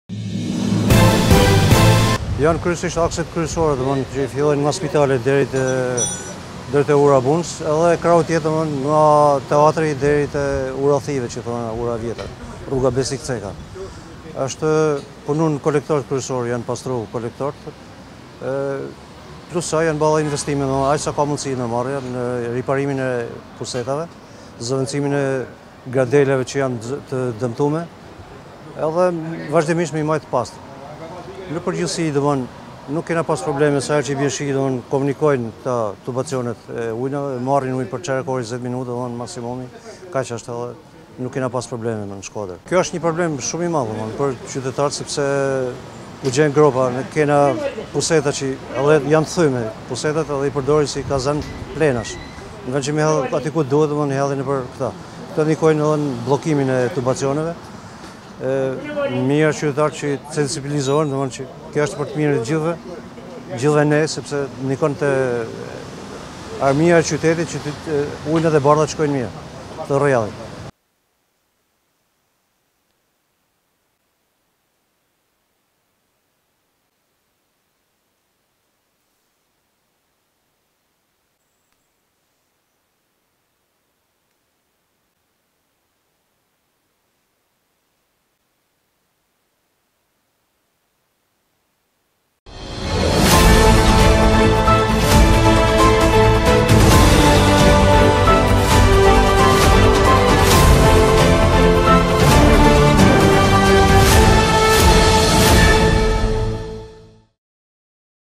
ShkodraWeb | Rreziku i përmbytjeve nga reshjet intensive, UK Shkodër pastron kanalizimet e KUB- së
Me qëllim minimizmin e pasojave që shkaktojnë reshjet e shiut në territorin e bashkisë Shkodër, ndërmarrja e ujësjellës kanalizime përmes sektorit i kanalizimeve të ujrave të bardha po vijon pa ndërprerje ndërhyrjet në të gjitha akset rrugore.

Gjatë të hënës janë kryer pastrimet e kanalizimeve dhe riparimet e dëmtimeve në bulevardin “Skenderbeg”, që përfshin segmentin rrugor nga sheshi “Demokracia” në drejtim të stacionit të Trenit.